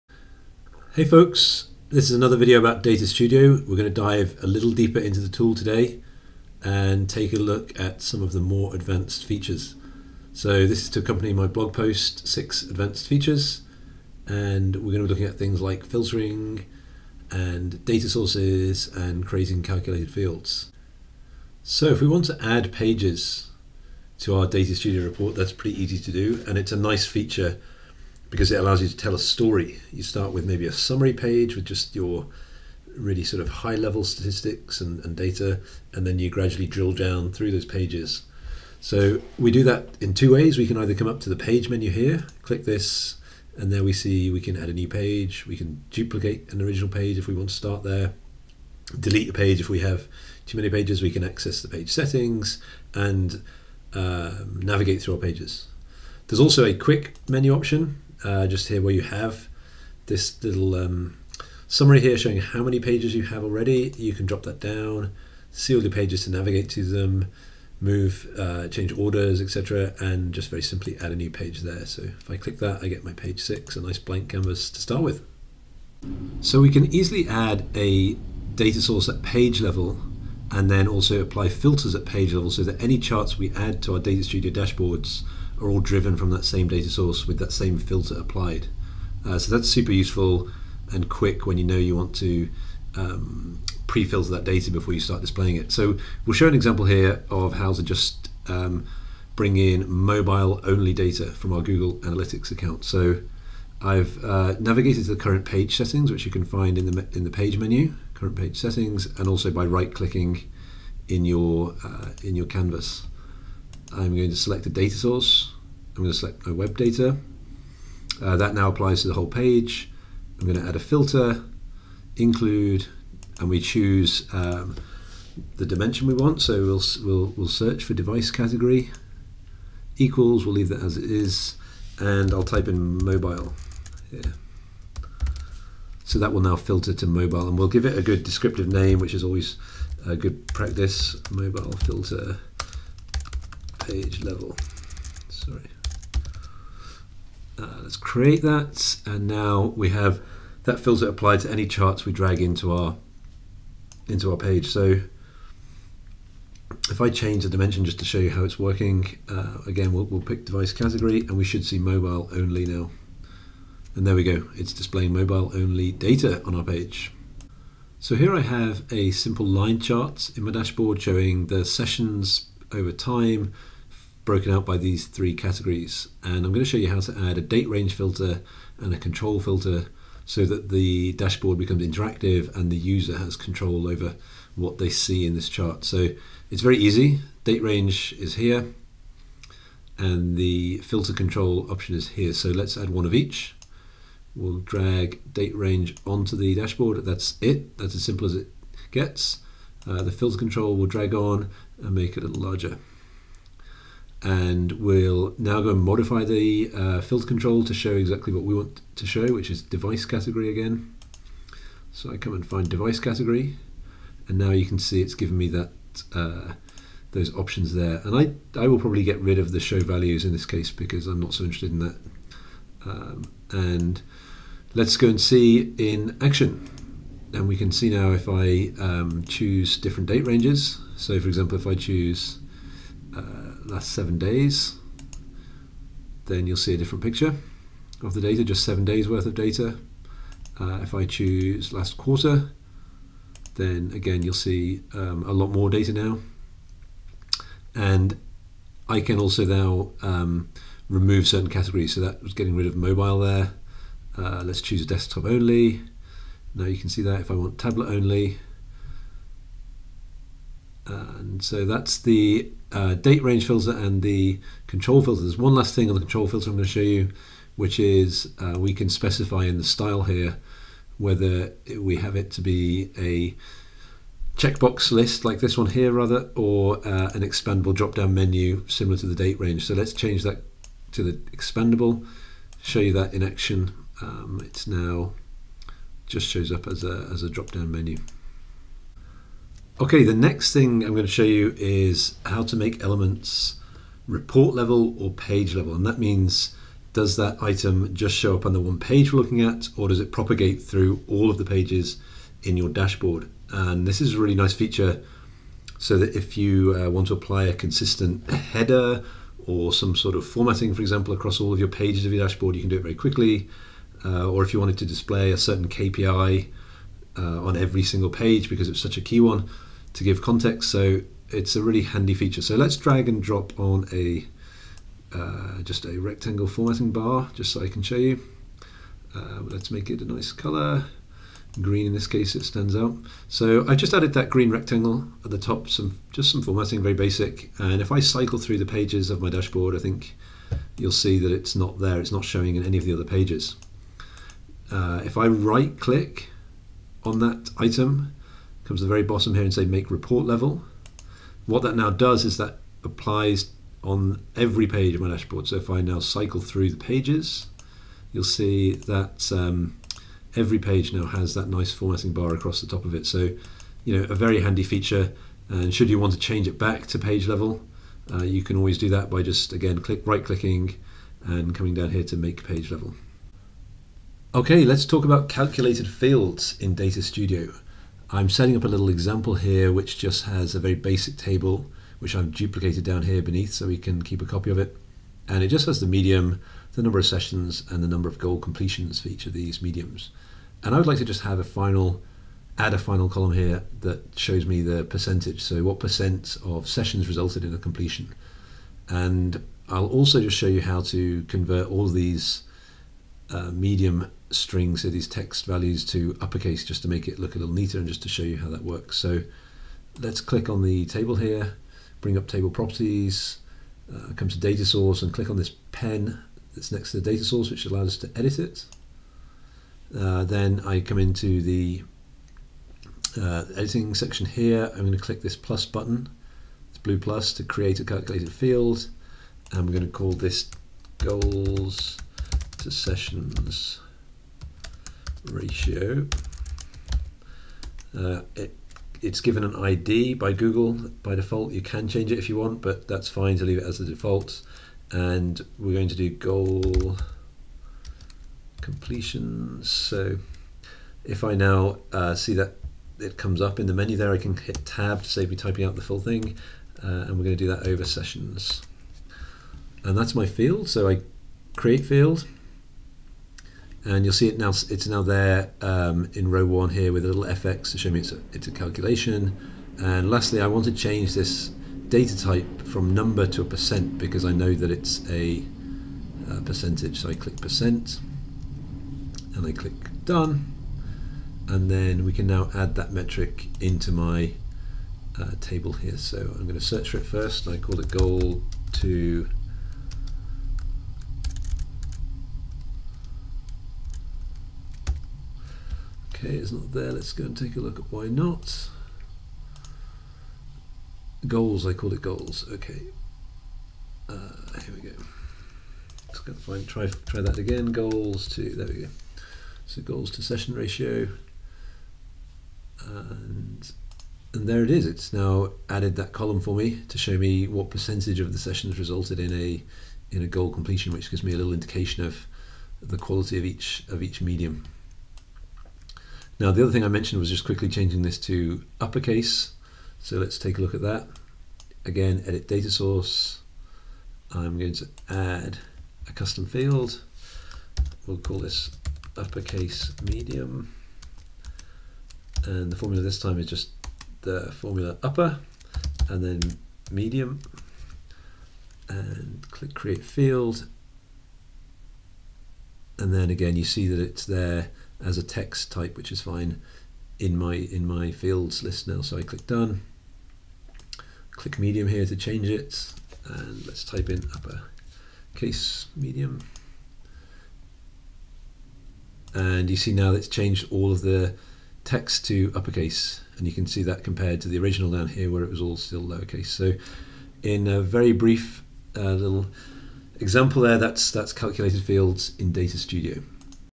6 advanced techniques in Google Data Studio
A close look at the Google Data Studio business dashboard reporting tool. In this video I look at 6 techniques to master for creating dashboard reports using this tool, including filtering and calculated fields.